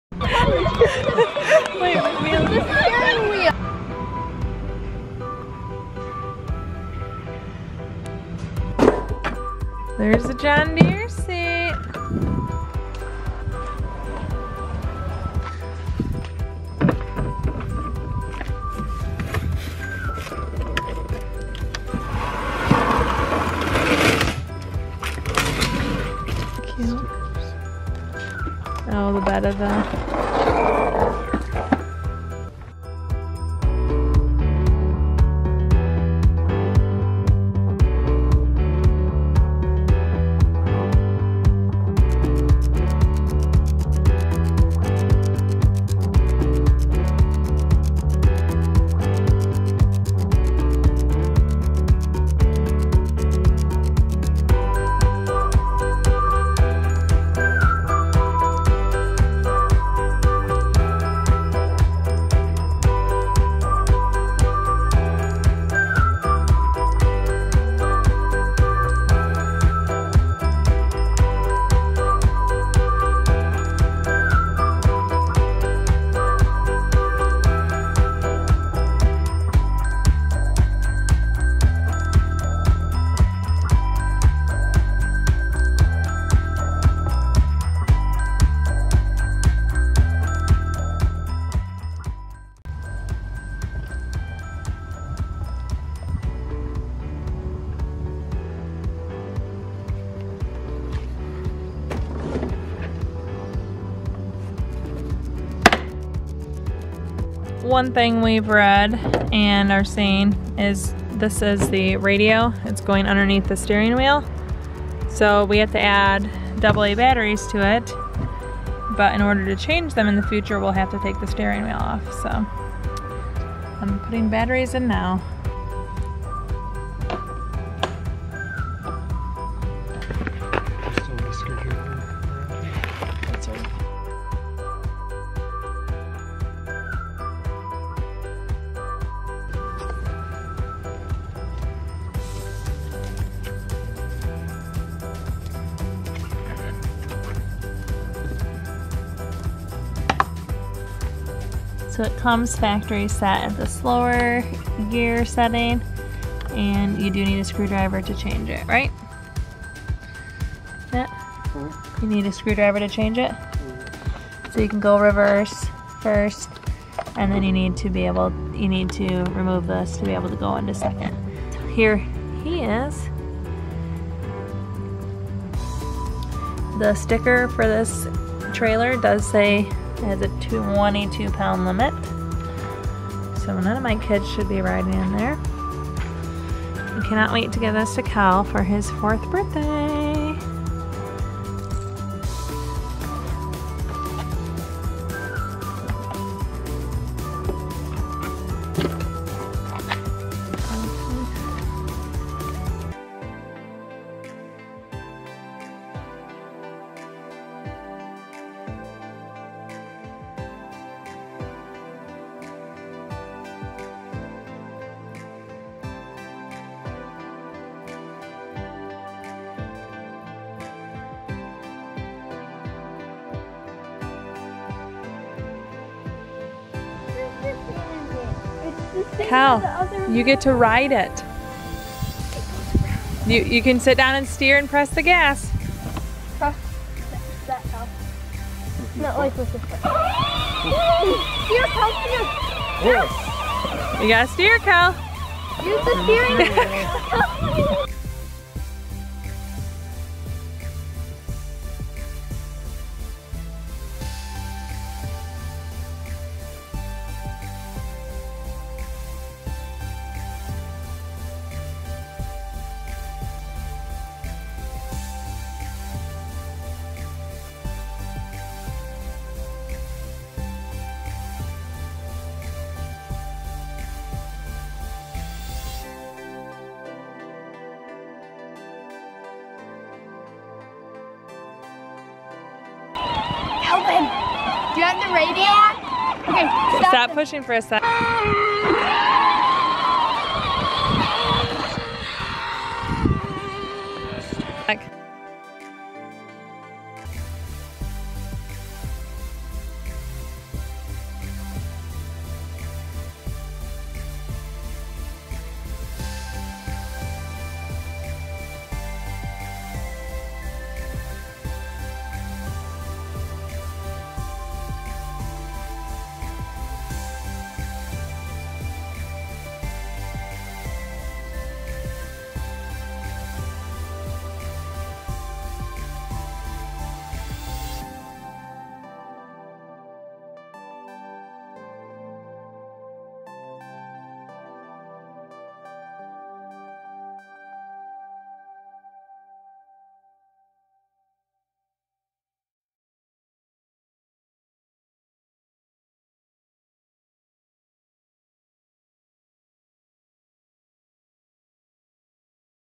Peg Perego 12v John Deere (worth it?!) Peg Perego Unboxing and Assembly | Toy Cars for kids
We put our son's new Peg Perego John Deere tractor with trailer (12v) together and share him taking it for a spin on his 4th birthday.  This 12v Peg Perego Ride on Tractor has a detachable trailer (22lbs weight limit), a working radio and two speeds along with reverse.

We purchased this from Target.  Also available at other retailers.

Matt (Dad) is always working on projects, so we wanted to get Kelvin a ride to help with all projects.

We are the Moravek Family.  On this channel we share reviews, overviews, uses, unboxings of products we use at home.